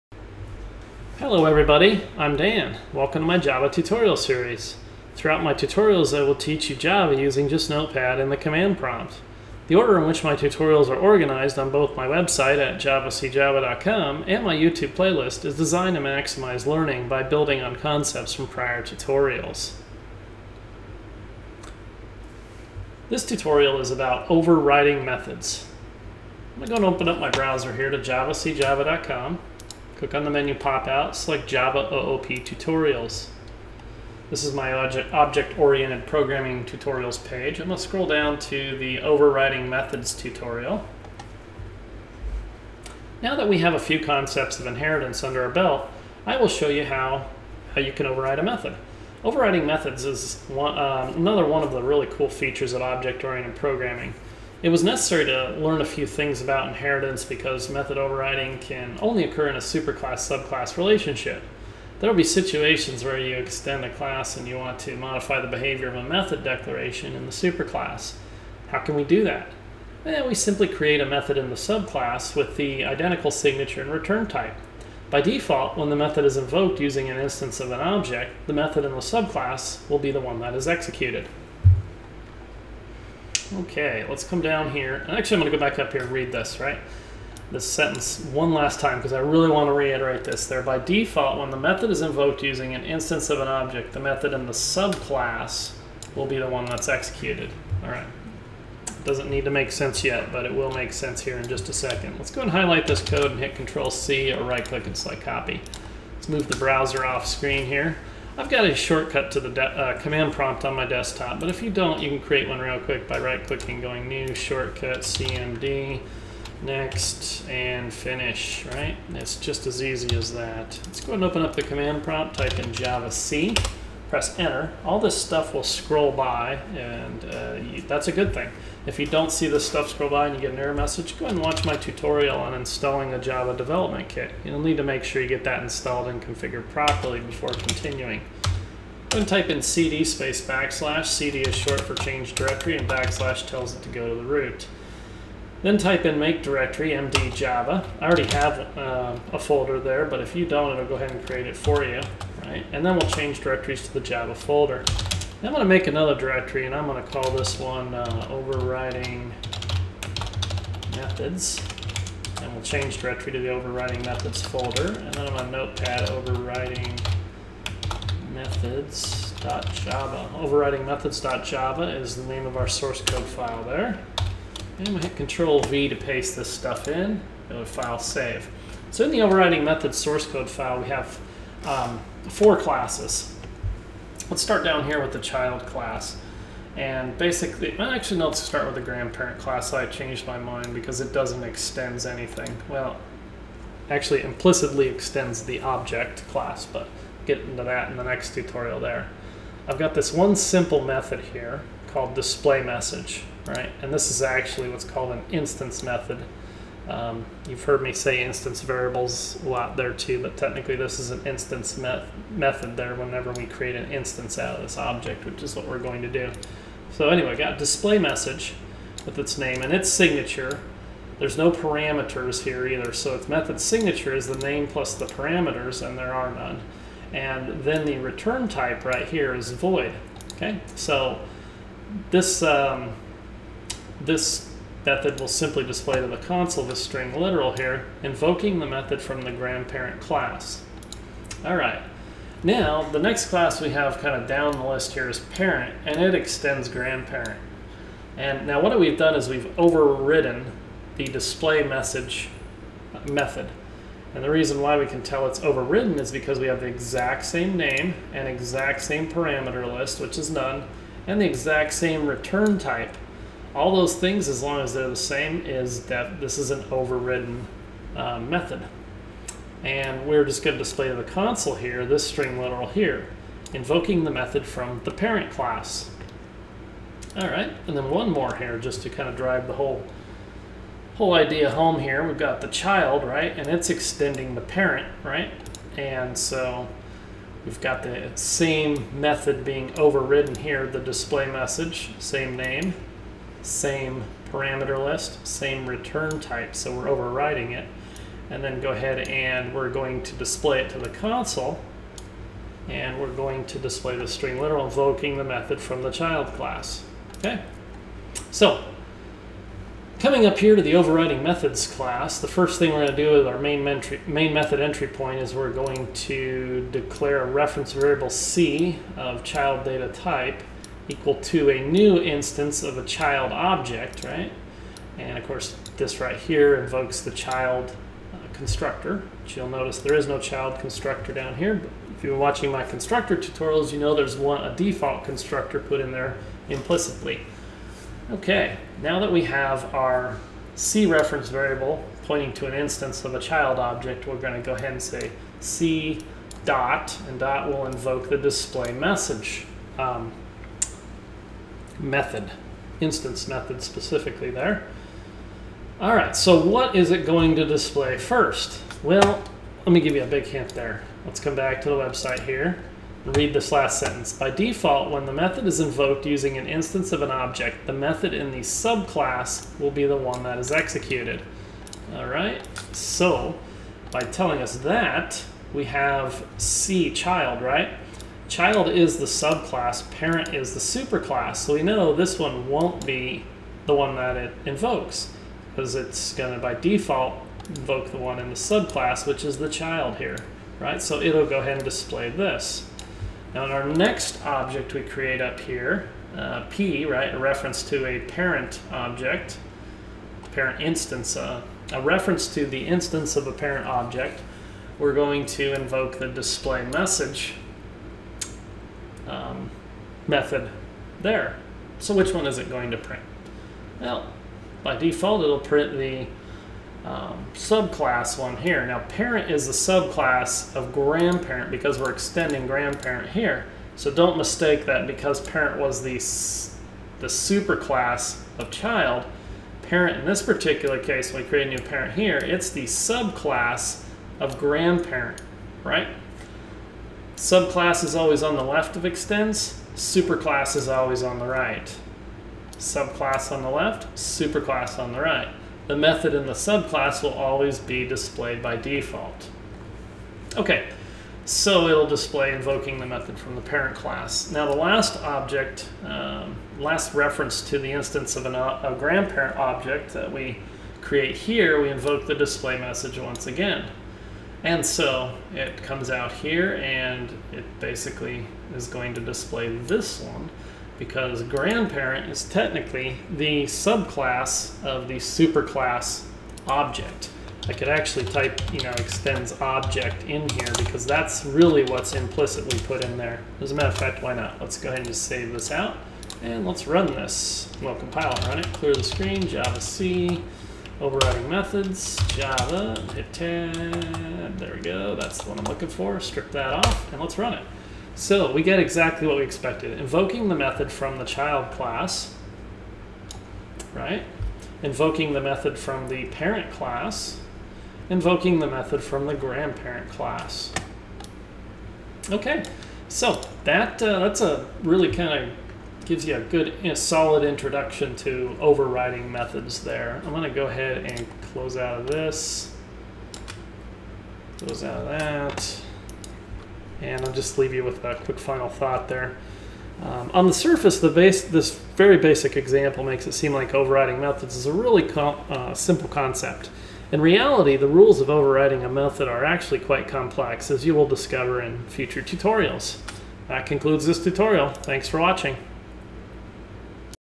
Learn Java Programming - Inheritance: Overriding Methods Tutorial
Now that we have a few concepts of inheritance under our belt, I will show you how you can override a method. Overriding methods is another one of the really cool features of object-oriented programming. It was necessary to learn a few things about inheritance because method overriding can only occur in a superclass/subclass relationship. There will be situations where you extend a class and you want to modify the behavior of a method declaration in the superclass. How can we do that? We simply create a method in the subclass with the identical signature and return type. By default, when the method is invoked using an instance of an object, the method in the subclass will be the one that is executed.